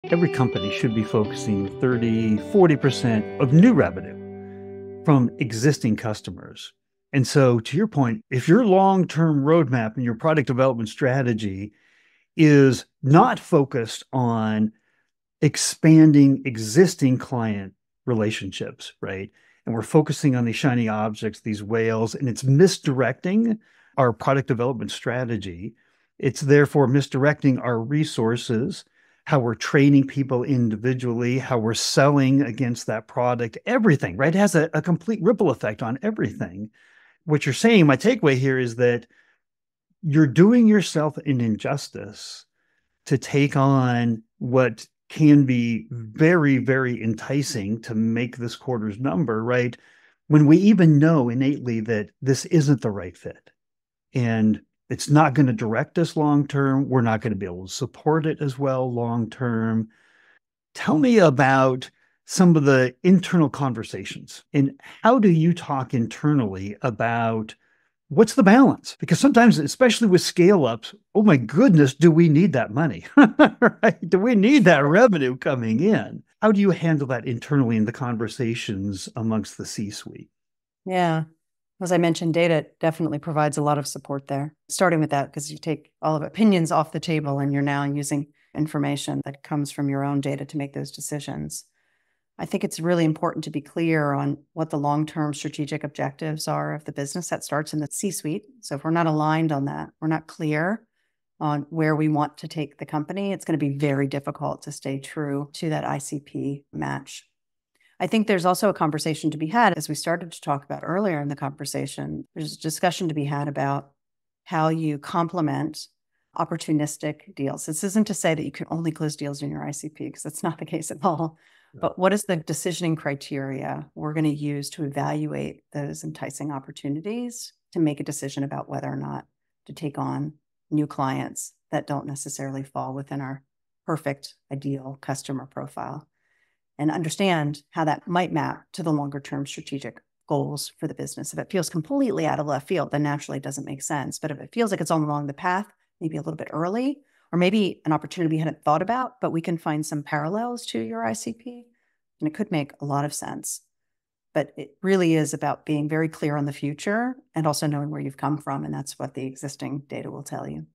Aligning C-Suite Conversations with Data-Driven Decisions
In this 2:40 video, Rhiannon Staples emphasizes using data to guide internal discussions and balance revenue needs with long-term goals. She highlights the importance of C-Suite alignment on strategic objectives and the role of data in evaluating opportunistic deals. She also underscores that clear criteria are essential for integrating new clients while staying true to the ICP.

“It’s really important to be clear on the long-term strategic objectives of the business, starting in the C-Suite. If we’re not aligned on that and not clear on where we want to take the company, it will be very difficult to stay true to the ICP match.” – Rhiannon Staples